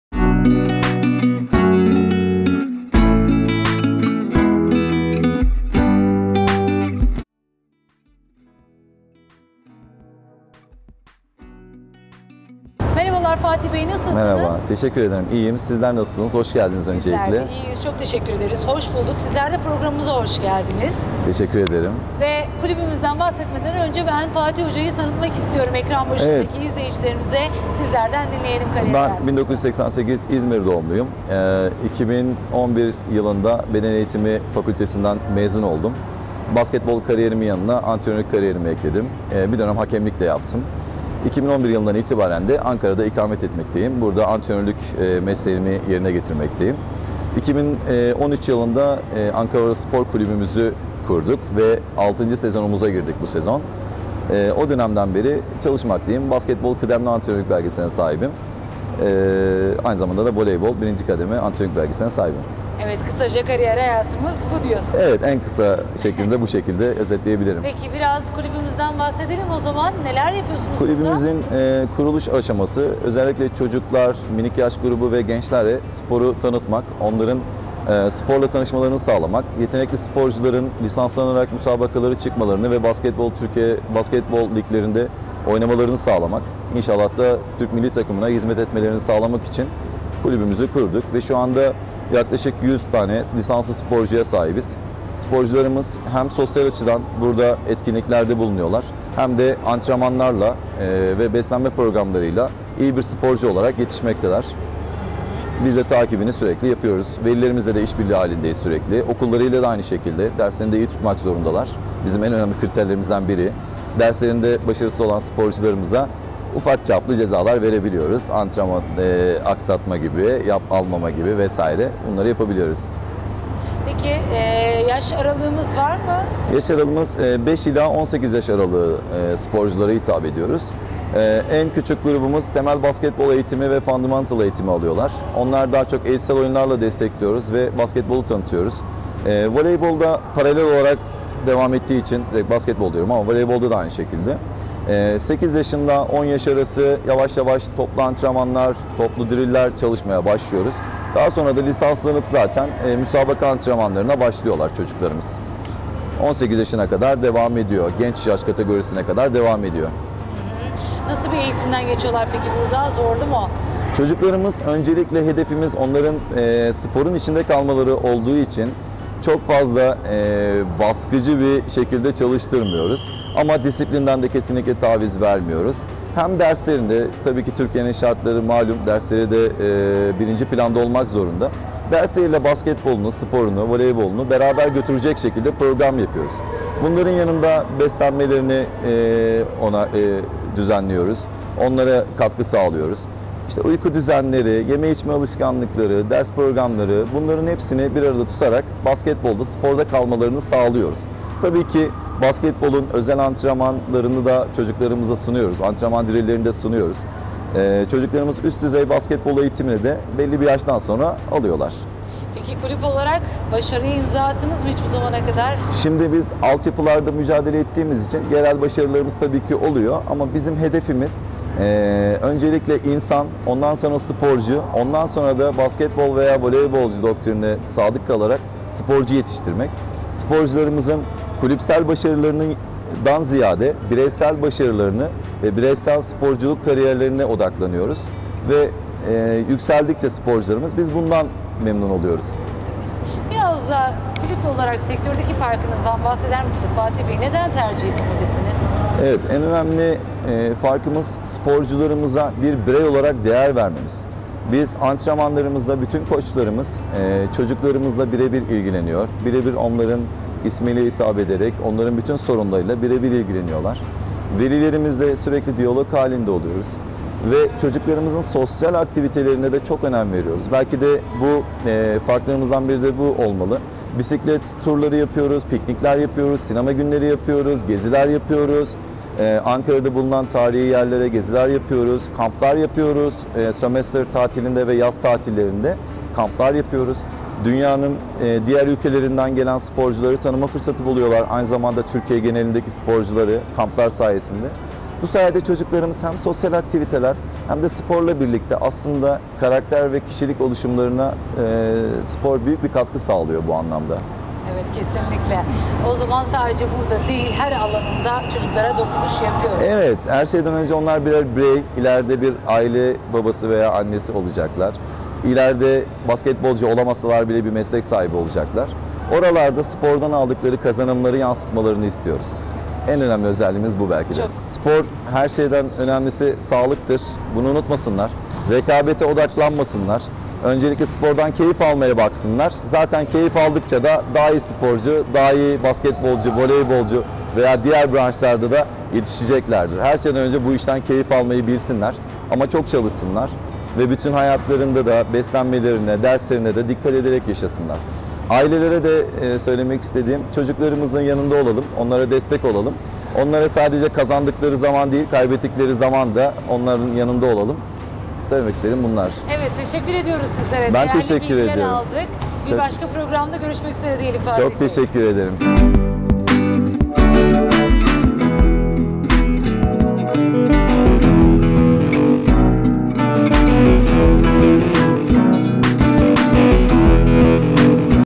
ANKARA SPOR MERKEZİ
Ankara Çankaya'da faaliyet gösteren 'Ankara Spor Merkezi' firma sahibi Fatih Mersin, sektördeki faaliyet ve başarılarını 'Bugün Kobi Özel'e anlattı.